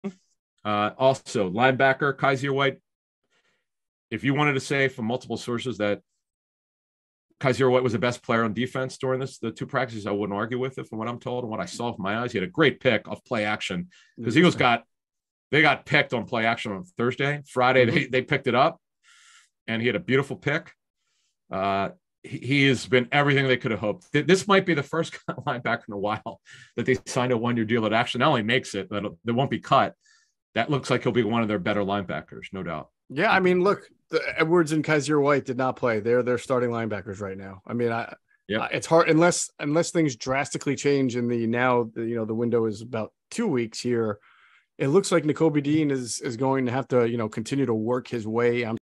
Philadelphia Eagles LB Kyzir White "Best Player On Defense" Vs. Cleveland Browns At Joint Practices
Geoff Mosher and Adam Caplan discuss the Eagles standouts from joint practices against the Cleveland Browns and the second preseason game.